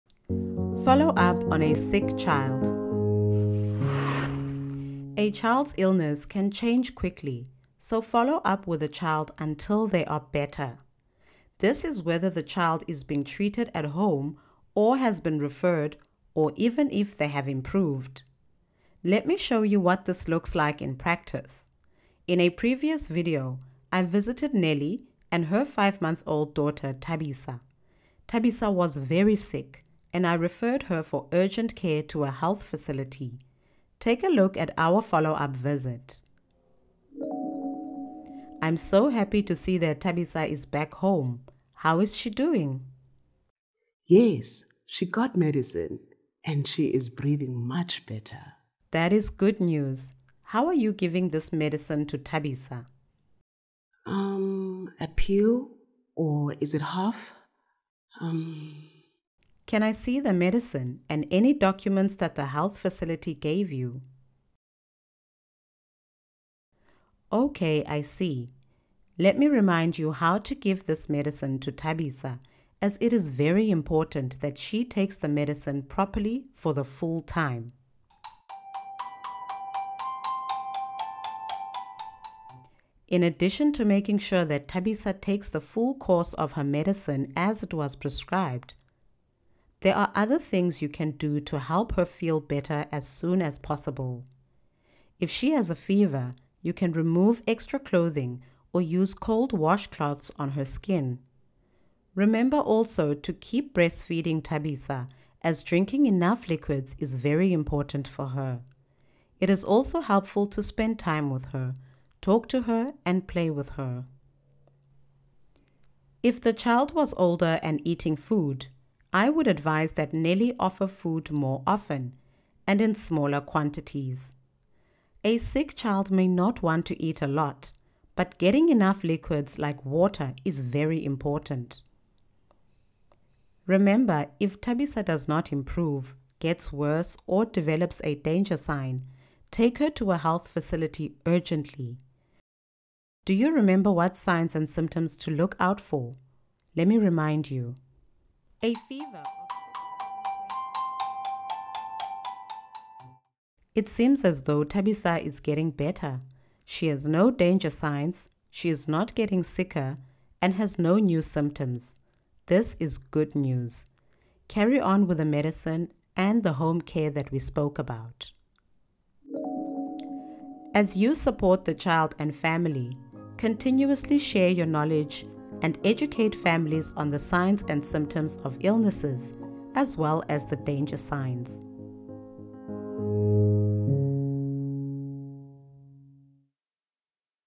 Follow Up on a Sick Child
A child’s illness can change quickly so follow up with the child until they are better. Explore how to do this whether the child is being treated at home or has been referred.